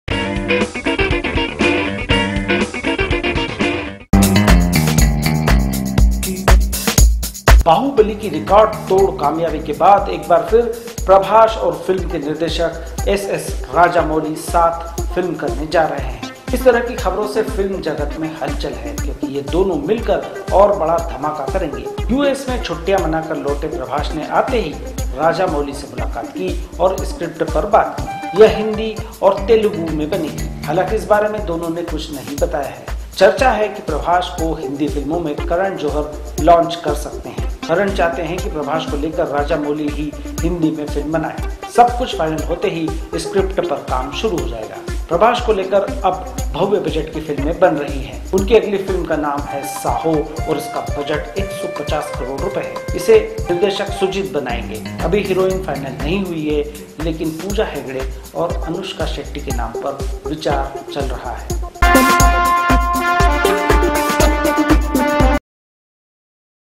अगली बाहुबली... प्रभाष और राजामौली की
बाहुबली की रिकॉर्ड तोड़ कामयाबी के बाद एक बार फिर प्रभाष और फिल्म के निर्देशक एसएस राजामौली साथ फिल्म करने जा रहे हैं। इस तरह की खबरों से फिल्म जगत में हलचल है क्योंकि ये दोनों मिल कर और बड़ा धमाका करेंगे। यूएस में छुट्टियां मना कर लौटे प्रभाष ने आते ही राजामौली से मुलाकात की और स्क्रिप्ट पर बात की।